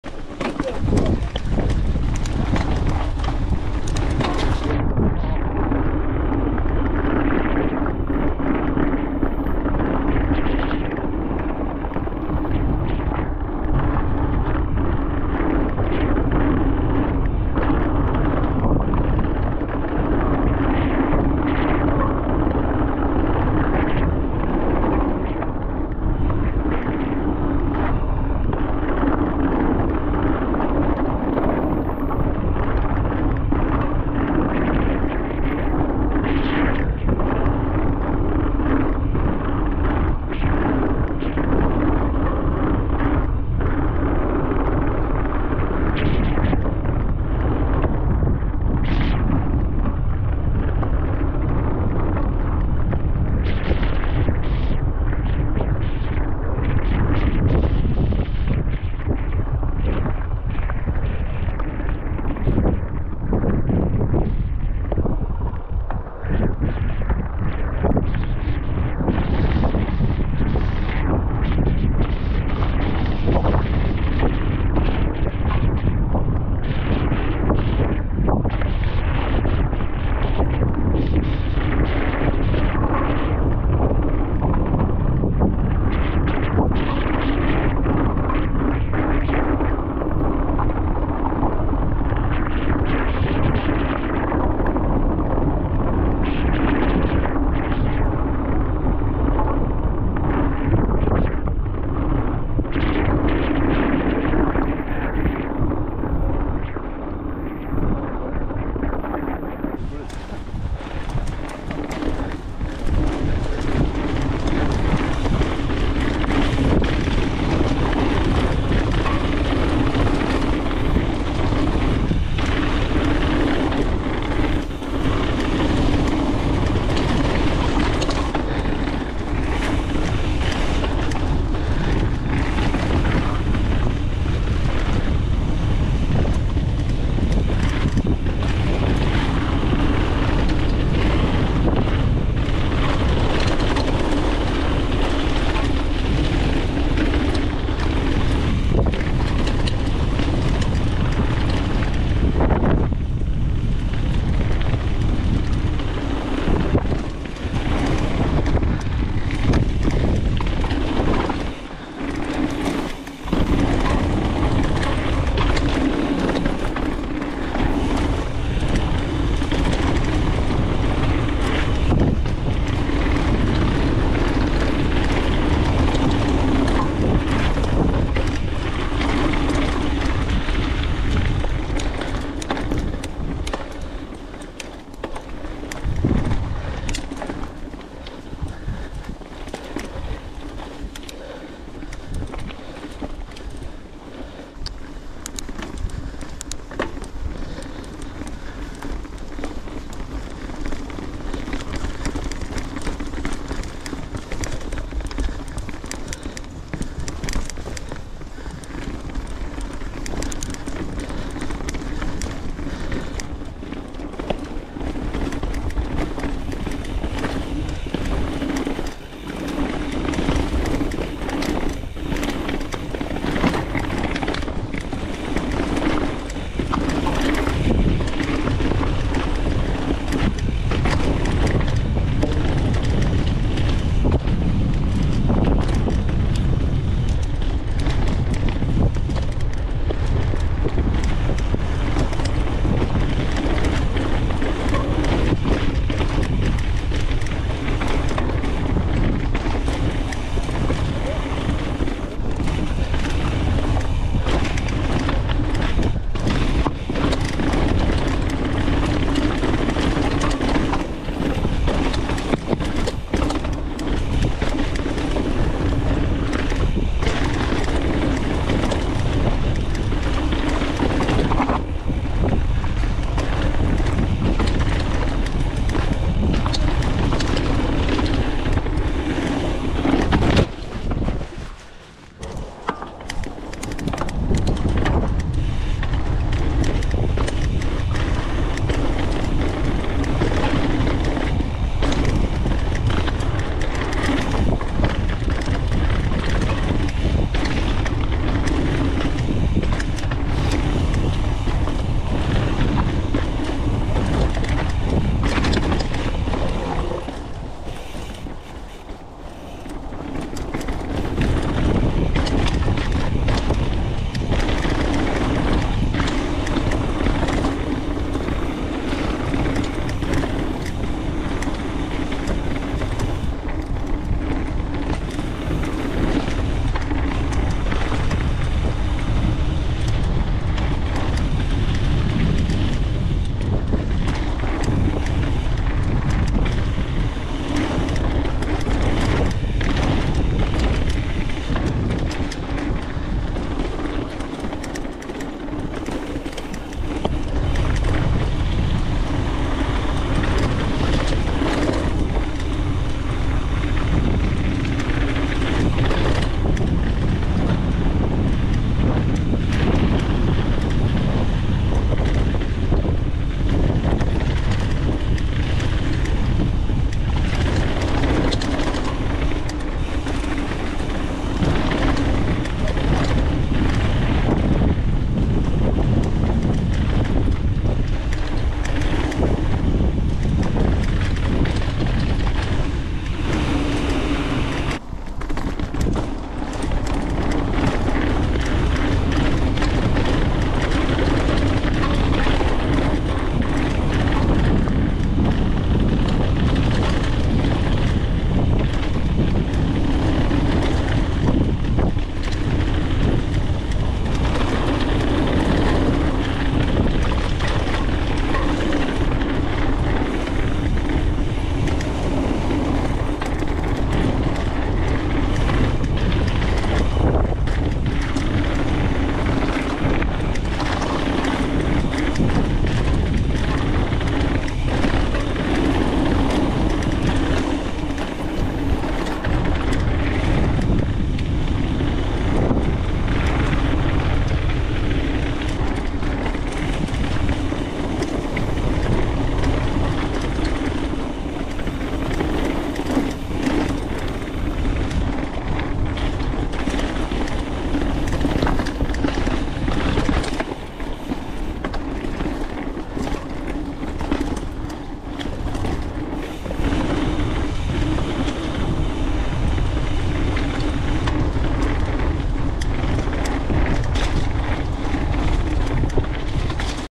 BIKE PARK WALES - Melted Welly into Surfin' Bird and Norkle | POV Run
A full run of Melted Welly into Vicious Valley and Norkle

GoPro Hero 11 Black - 4K 30FPS

Location: Bike Park Wales

Bike : Specialised Stumpjumper

-- Social Media --